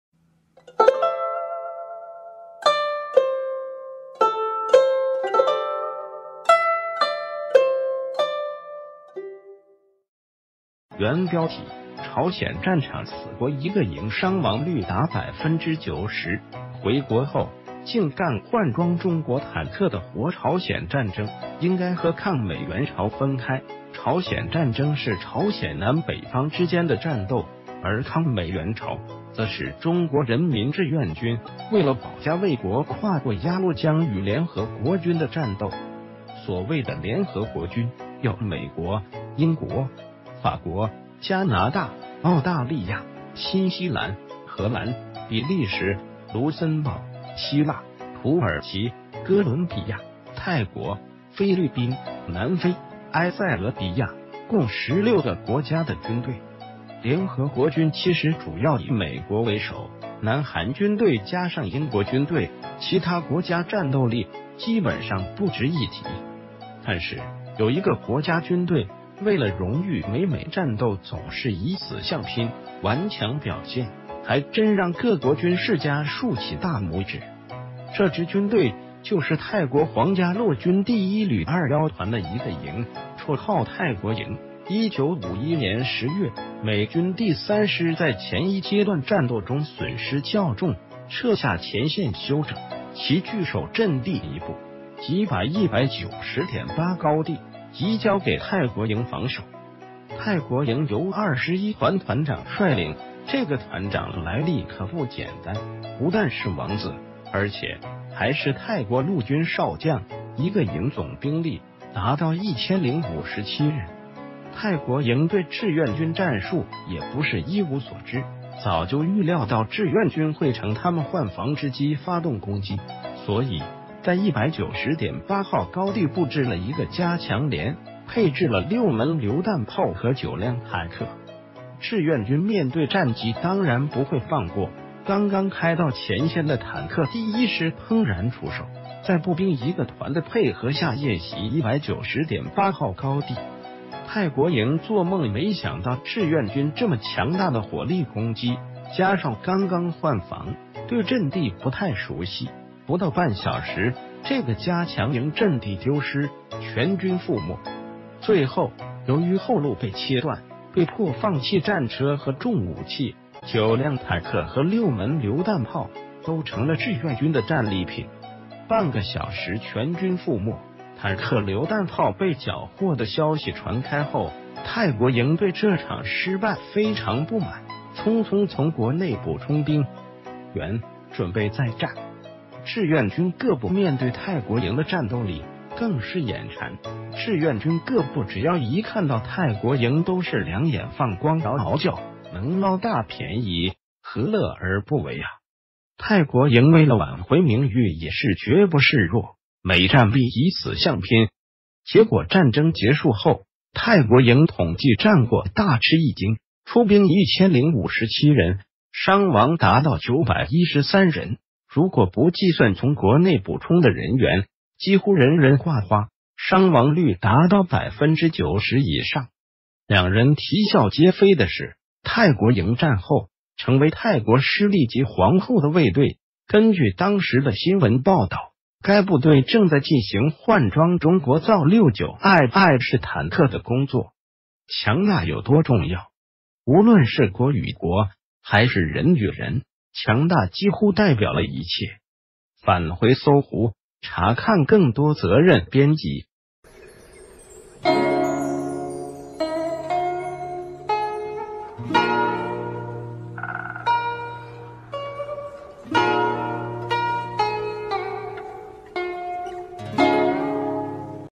朝鮮戰場此國一個營傷亡率達90%，回國后竟干換裝中國坦克的活_戰斗
原標題：朝鮮戰場此國...